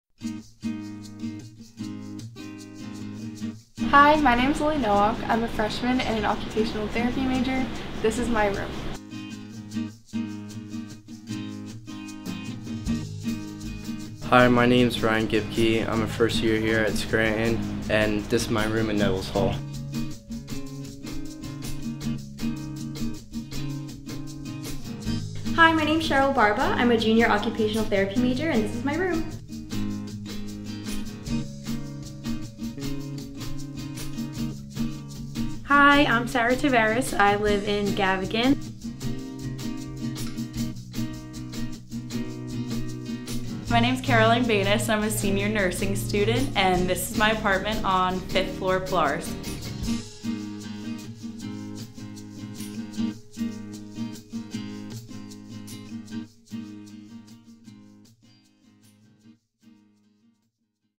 Residence Hall Tour
Take a look at how University students decorate their Residence Halls!

Video by Maxwell Earl '21, a journalism and electronic media major.